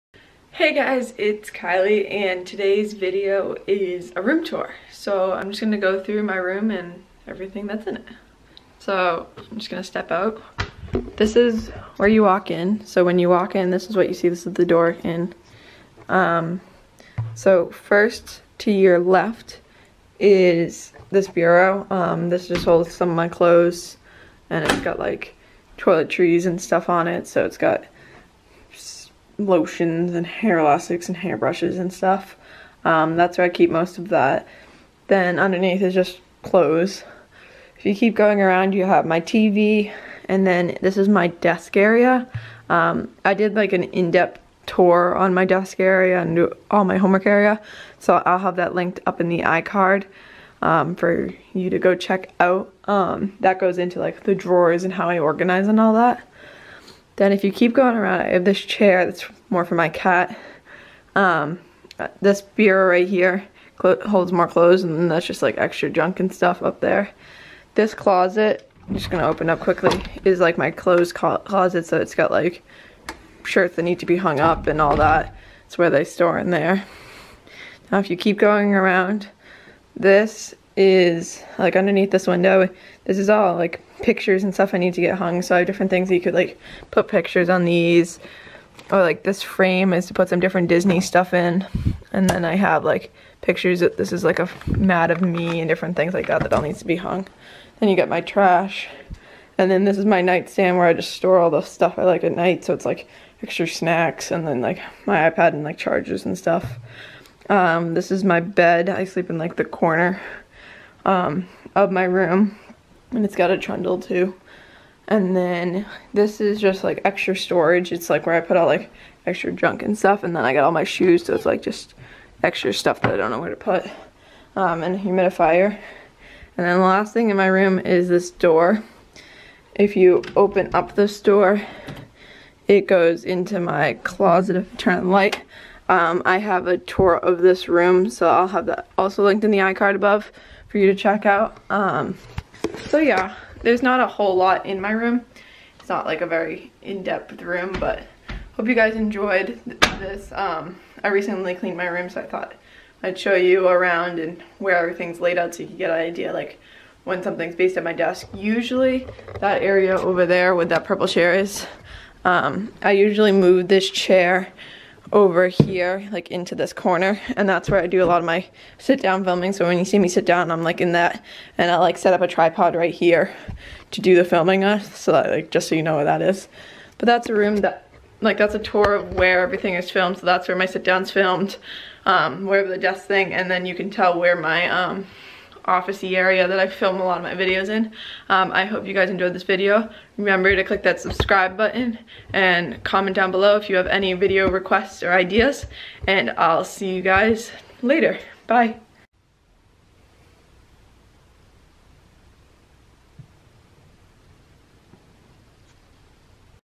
Room Tour | January 2018
This is a tour of my room. This allows you to get a feel of where I do my filming and work.